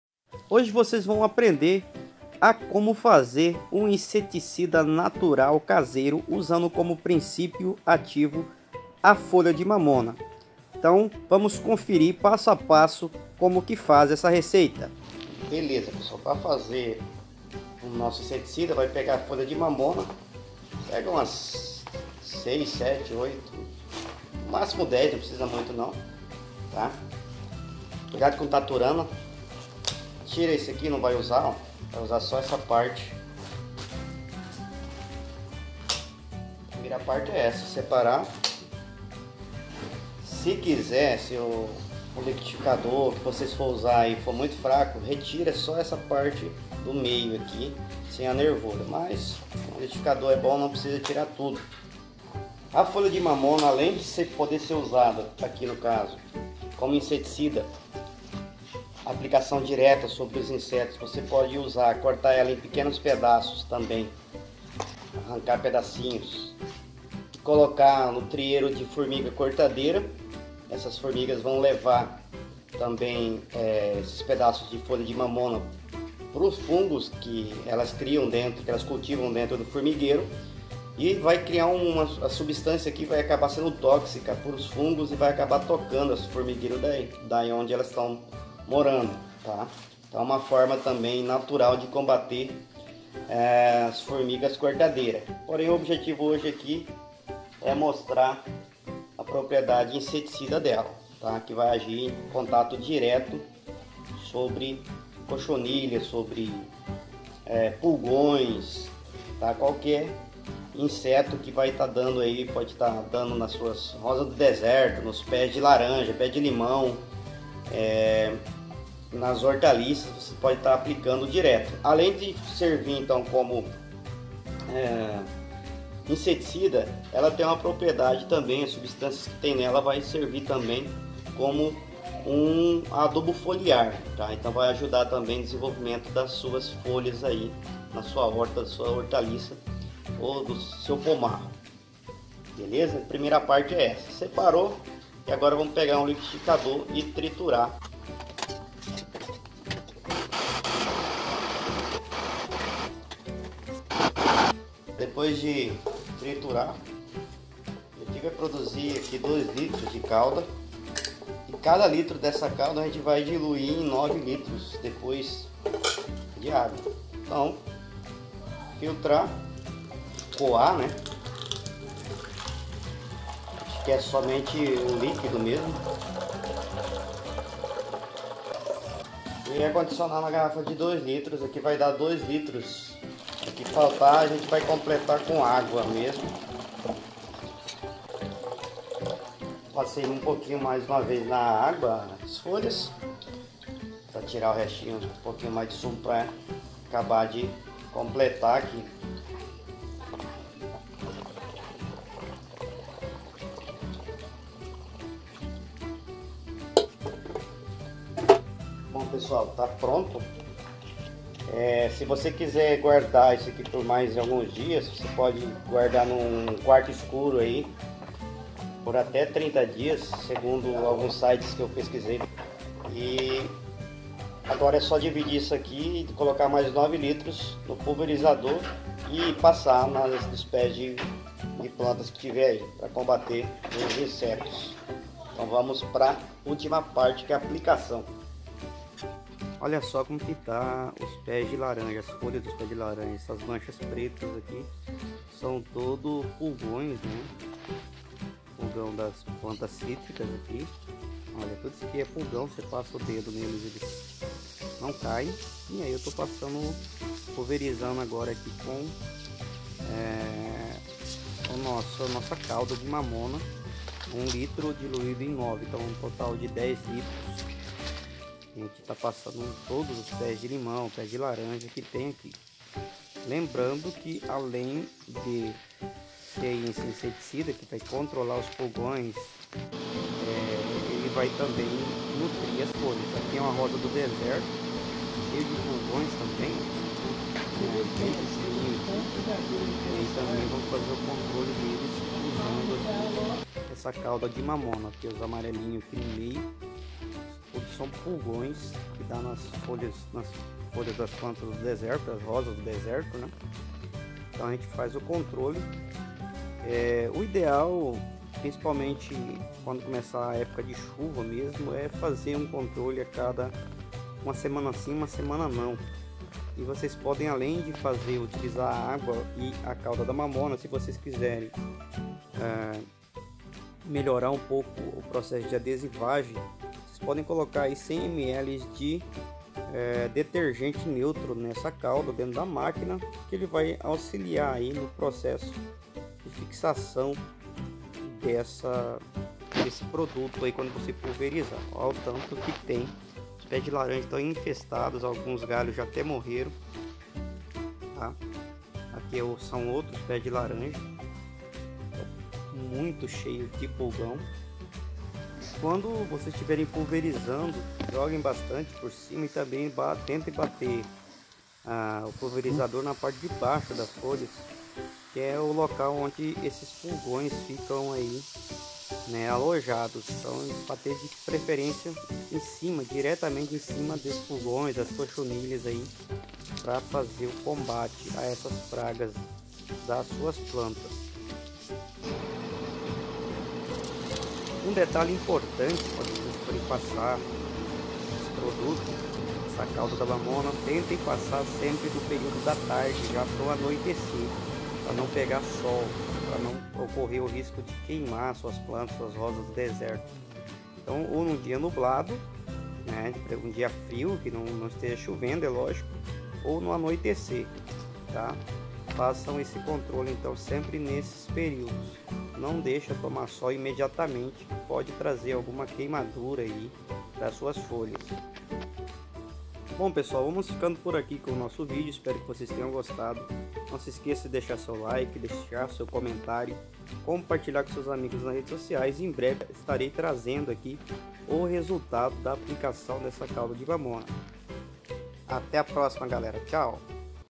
INSETICIDA CASEIRO com Folha de MAMONA | Faça seu próprio inseticida caseiro
Como Fazer INSETICIDA CASEIRO NATURAL com Folha de MAMONA
Neste vídeo você irá aprender a fazer um INSETICIDA CASEIRO NATURAL com folha de mamona.
Colete 10 folhas de mamona retire o talo. Pegue um liquidificador e coloque 1 litro e meio de água, em seguida coloque de três em tres folhas dentro e liquidifique, acrescentando todas as folhas.
Depois de bater as folhas de mamona no liquidificador, coe para separar o líquido das folhas.
Depois de coar acrescente água para completar 2 litros de cauda de folha de mamona.

Para cada litro de cauda de folha de mamona misture no máximo 9 litros de água para pulverizar suas plantas diretamente sobre os insetos.

Essa cauda de folhas de mamona serve tanto como INSETICIDA quanto ADUBO FOLIAR.

Realize a pulverização com a cauda de folha de mamona, o inseticida NATURAL caseiro sempre ao intardecer. Evite pulverizar o INSETICIDA de folha de mamona no momento de SOL FORTE para não ocorrer queimaduras nas plantas.